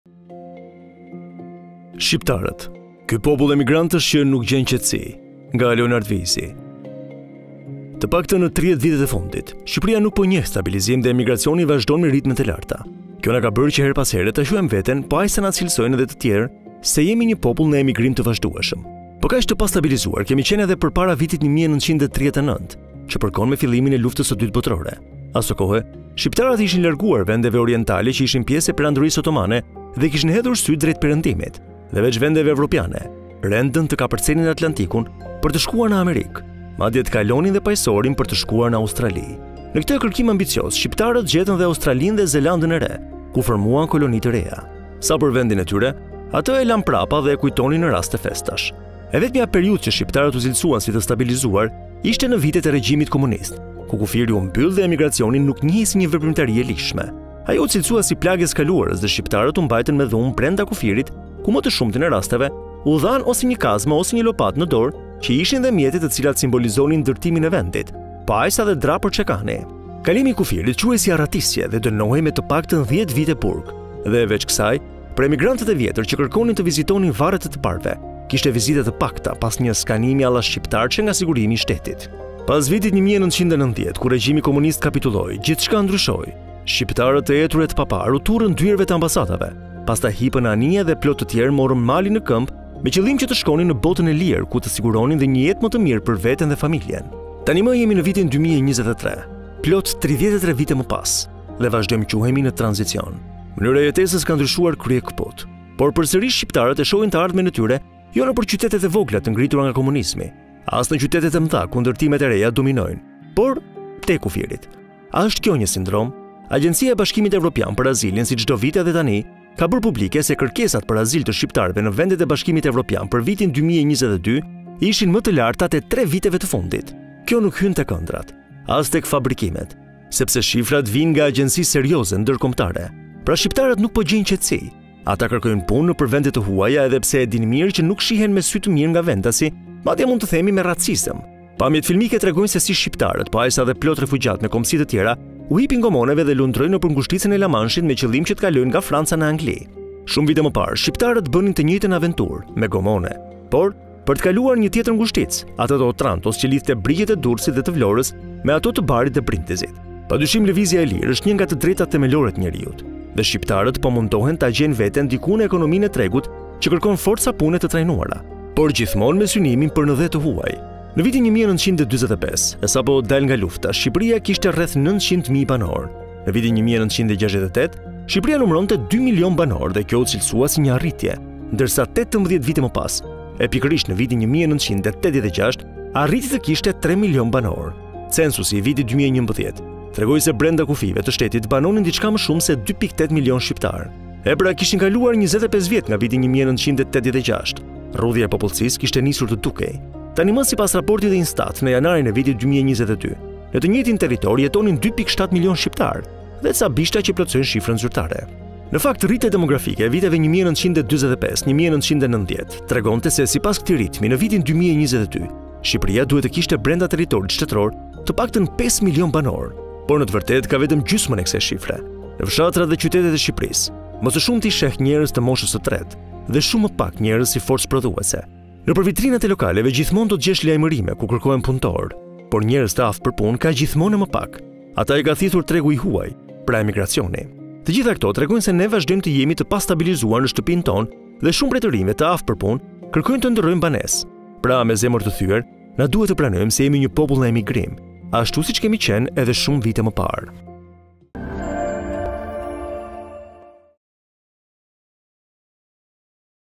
Shqiptarët, ky popull emigrantësh që nuk gjen qetësi/ Nga Leonard Veizi
Të paktën në 30 vitet e fundit Shqipëria nuk po njeh stabilizim dhe emigracioni vazhdon me ritme të larta. Kjo na ka bërë që herë pas here ta quajmë veten, - po aq sa na cilësojnë edhe të tjetër, - se jemi një popull në emigrim të vazhdueshëm. Po kaq të pastabilizuar kemi qenë edhe përpara vitit 1939 që përkon me fillimin e Luftës së Dytë Botërore. Aso kohe shqiptarët i ishin larguar vendeve orjentale që ishin pjesë e Perandorisë Otomane dhe kishin hedhur sytë drejt Perëndimit. Dhe veç vendeve evropiane, rendën të kapërcenin Atlantikun për të shkuar në Amerikë, madje të kalonin dhe Paqësorin për të shkuar në Australi. Në këtë kërkim ambicioz shqiptarët gjetën dhe Australinë e Zerlandën e Re ku formuan koloni të reja. Sa për vendin e tyre, atë e lanë prapa dhe e kujtonin në raste festash.